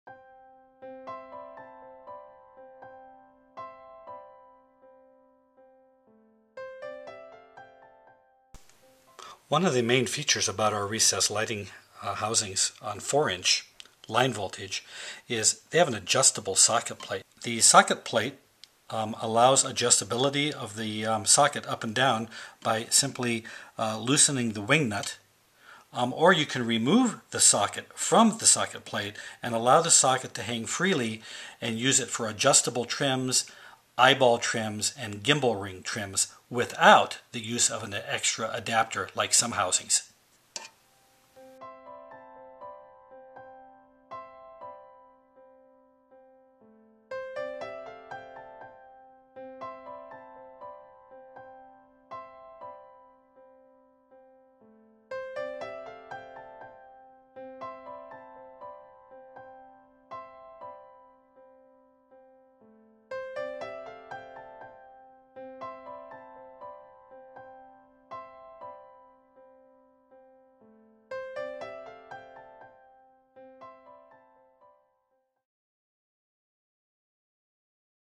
4" line voltage recessed housing features explained by Total Recessed Lighting
Here at Total Recessed Lighting, we are on a mission to demystify lighting one bulb and one fixture at a time!
Learn the many added benefits and features of our 4" line voltage recessed housings that separate us from standard 4" housings you typically find at home improvement centers and discount warehouses.